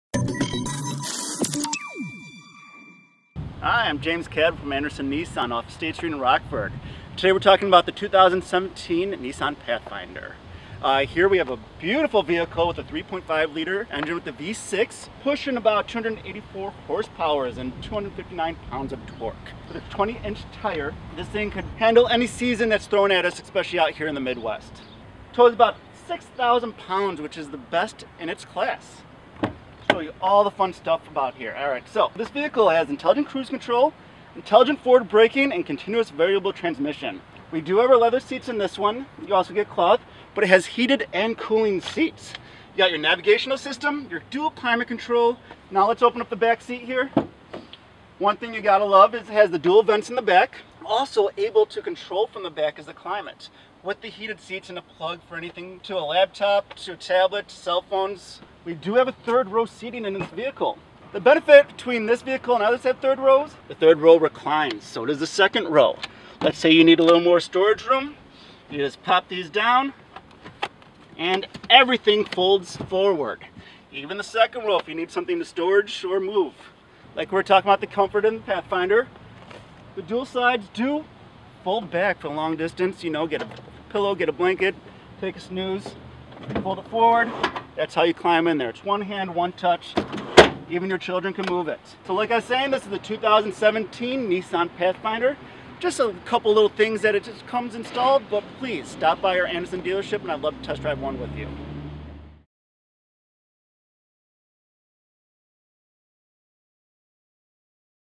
2017 Nissan Pathfinder Features at Anderson Nissan Rockford, IL
James Cadwell of Anderson Nissan in Rockford, IL shows us around a new 2017 Nissan Pathfinder. We learn about the powerful engine, CVT transmission, third row seating and other great features.